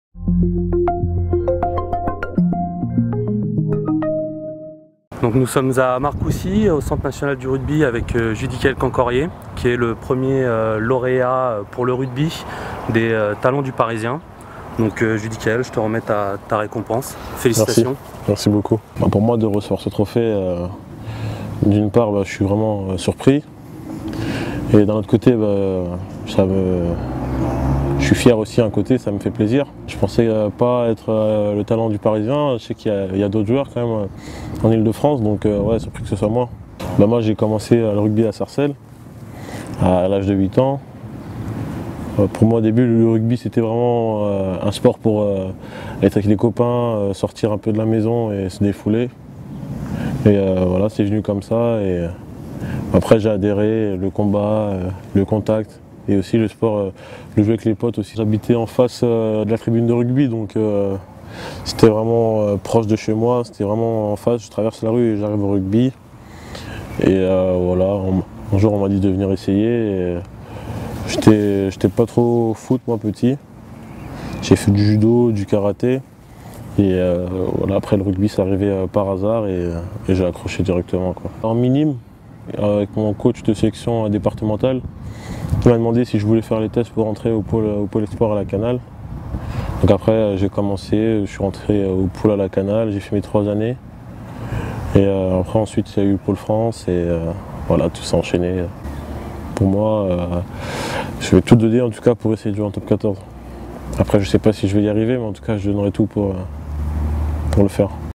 Talents du Parisien : Judicaël Cancoriet le nouvel espoir du rugby francilien
Judicaël Cancoriet a débuté le rugby à Sarcelles avant de rejoindre Massy en 2011. Auteur d'une saison pleine en Pro D2 il est sur le point de rejoindre l'un des clubs majeurs du Top 14. Capitaine des moins de 19 ans tricolores, il est déjà surclassé avec les moins de 20 ans, et disputera la Coupe du monde début juin en Italie).